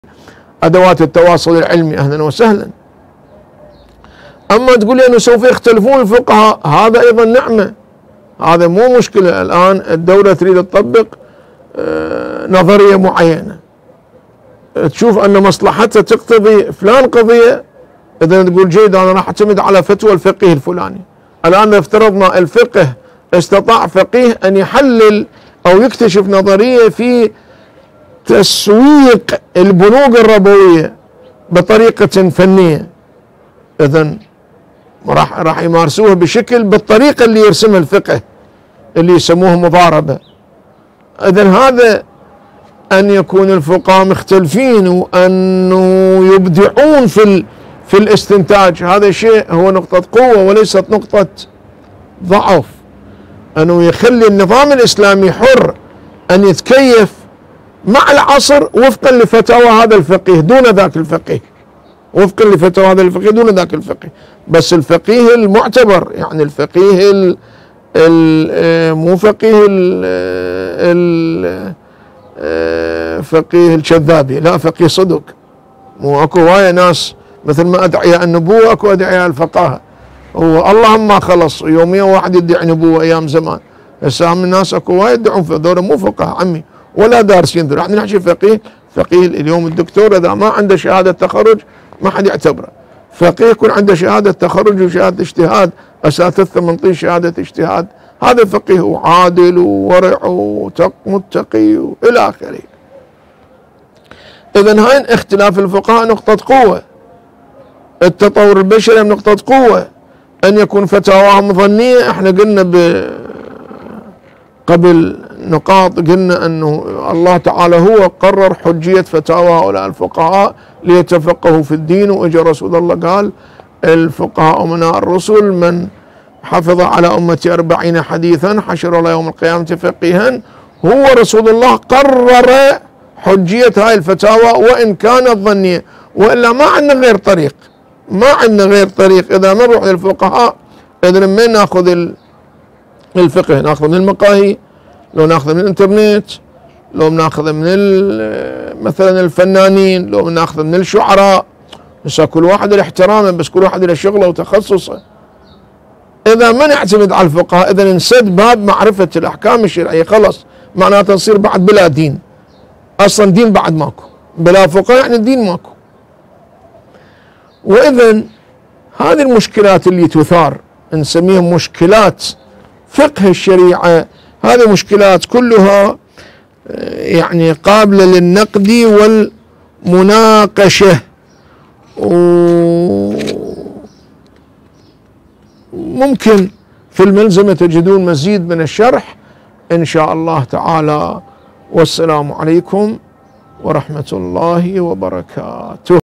المحاضرة الثامنة الدفاع عن الشريعة
سماحة السيد صدر الدين القبنجي
الجامعة الاسلامية في النجف الاشرف
الدفاع عن الشريعة
الجامعة الاسلامية في النجف الاشرف  Channel Islamic University